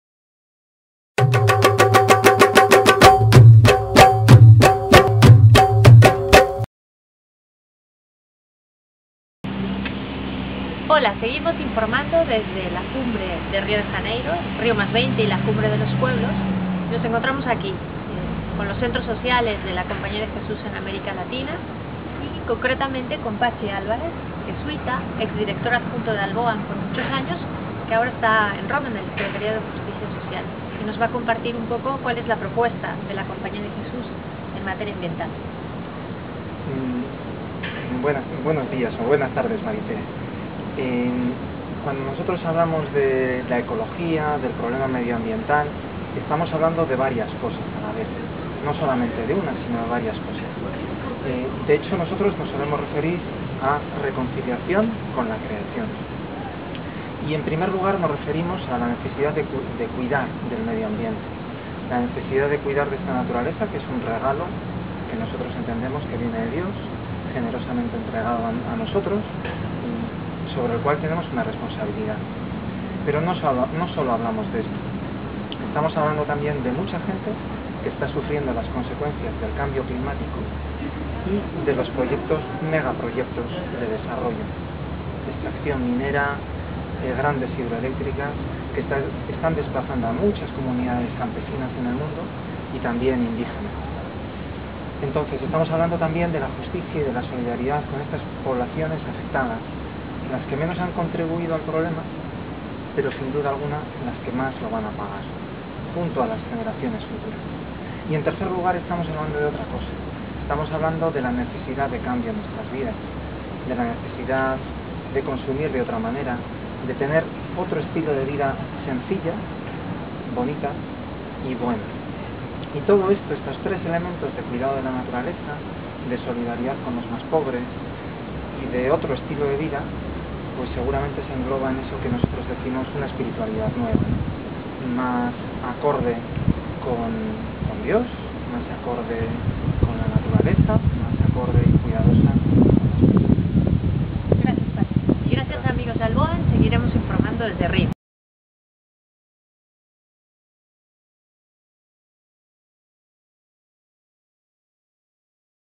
Crónica Río+20 2
Mary Tere Guzmán de ALBOAN y Patxi Álvarez de los Mozos del Secretariado de Justicia Social y Ecología de la Compañía de Jesús charlan sobre la importancia del cuidado de la naturaleza.